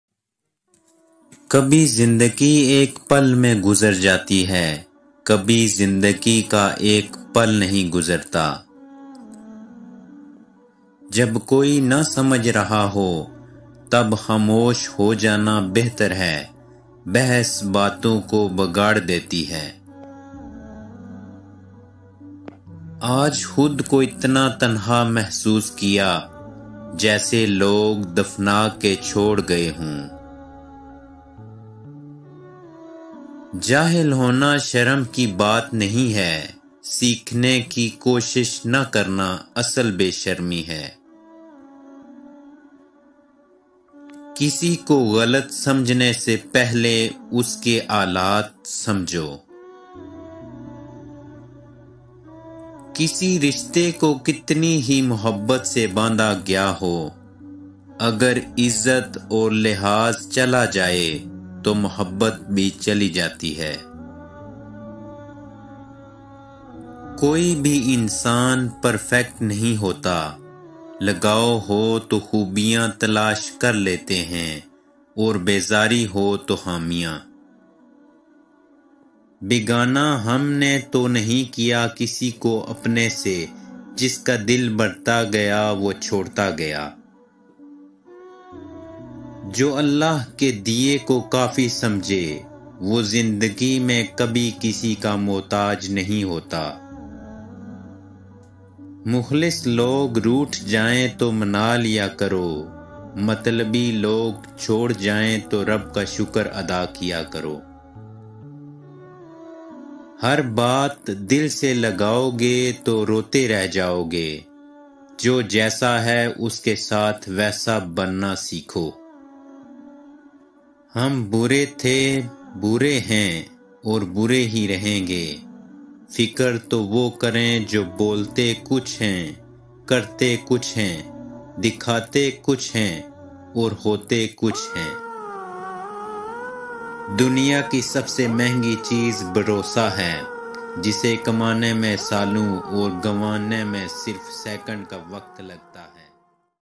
Jb koi na samj RHA ho ll Best collection of quotes ll Best voice creations
AOA
 In this video best collection of Islamic quotes for life changing. Very informative words.

Quotes, info, information, aqwal, qol, motivation, motivational, Urdu, Hindi, lyrics, best,